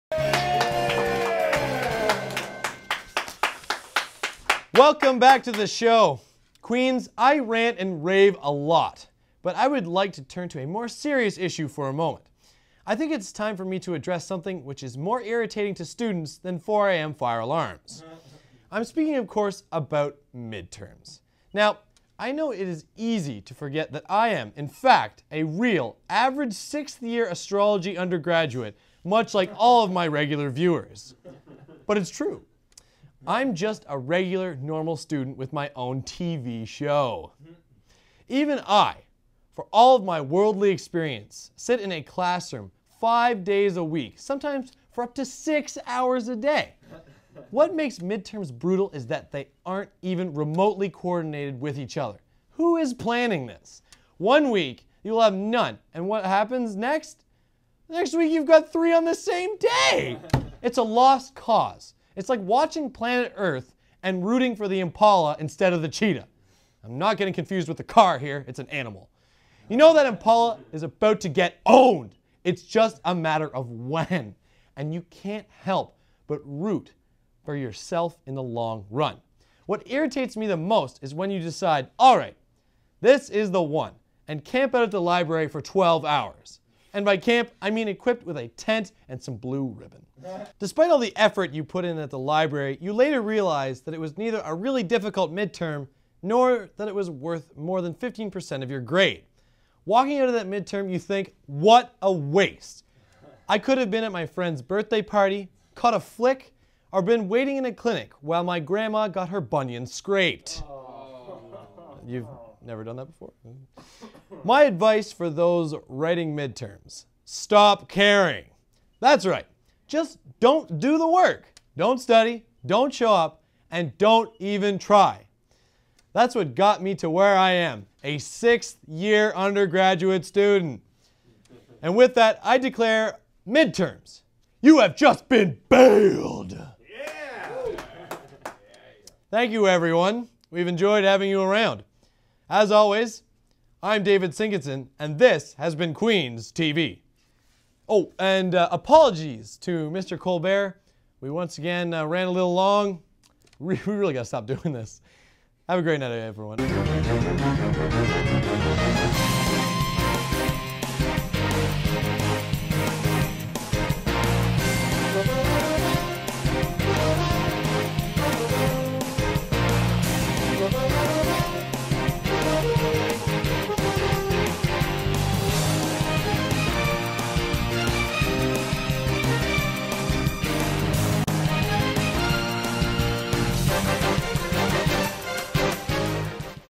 Why we Hate Mid-Terms- S 22- Ep 5 - Part 8
Tis' the Season: To stress out, study too much, and hate school. We send a return volley at this age-old, hated practice